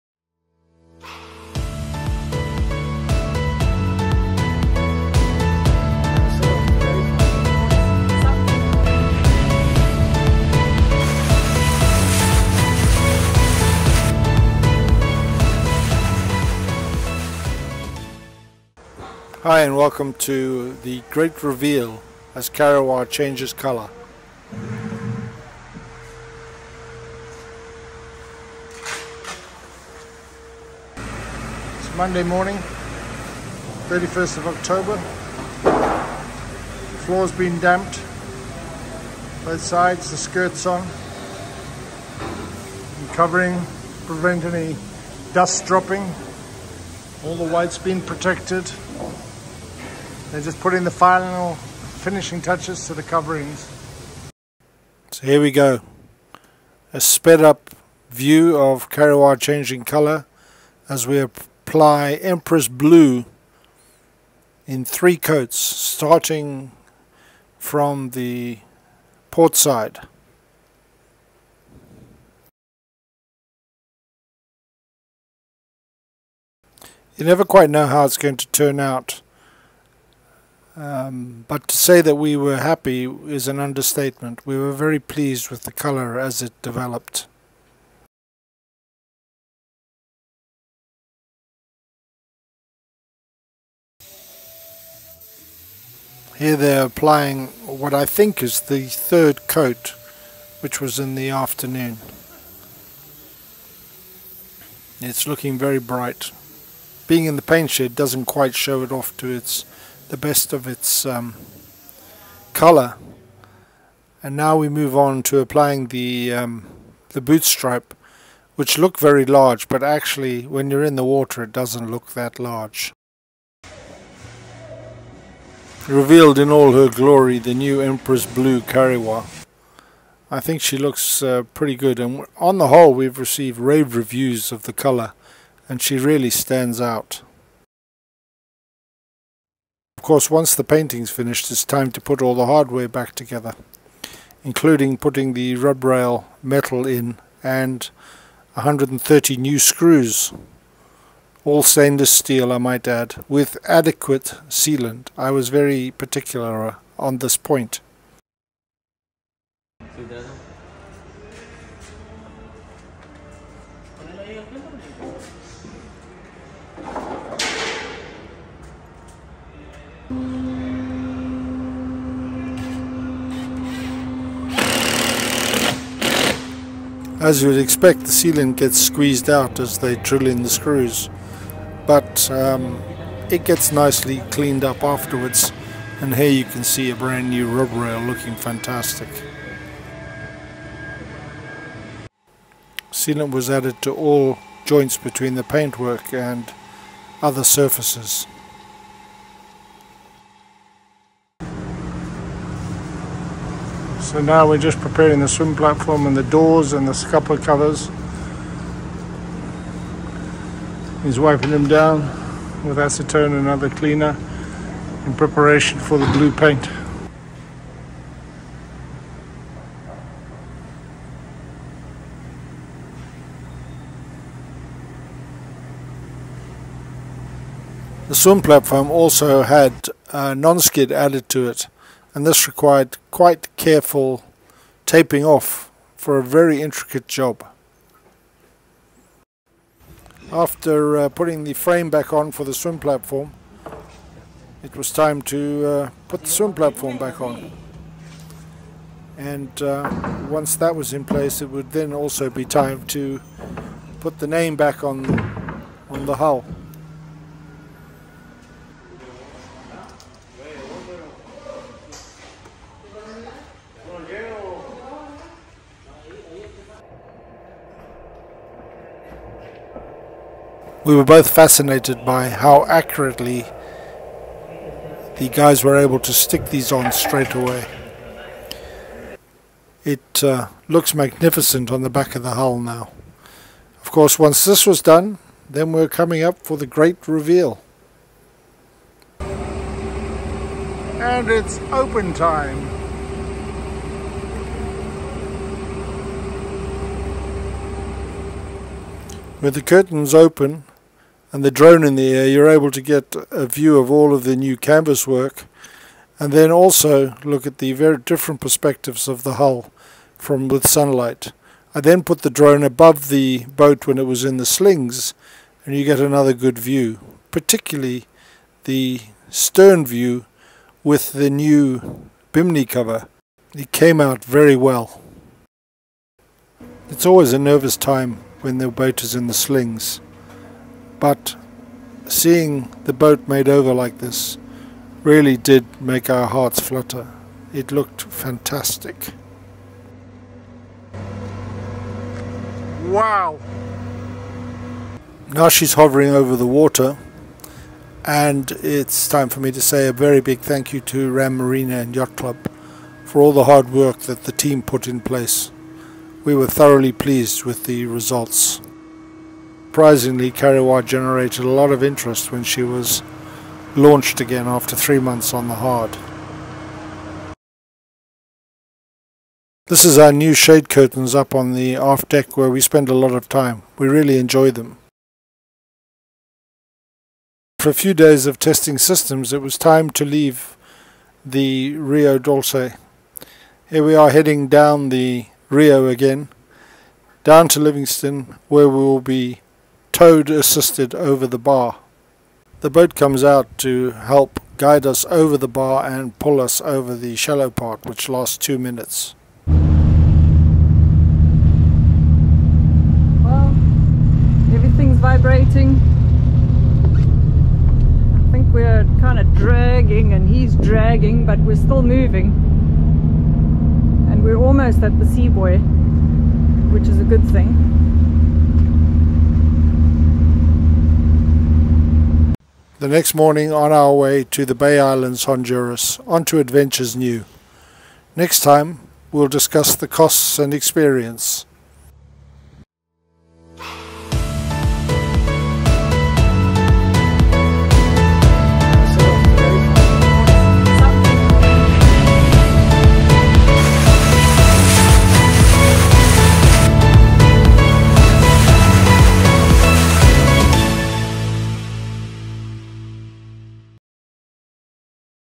Revealing the new color of Kariwa, the complete makeover of our iconic Nordie 62 HD 1080p
After 3 months on the hard and in the paint shed at RAM marina and yacht club, Río Dulce, Guatemala... We paint the hull a new color, put the hardware back on and get ready to start our next adventures.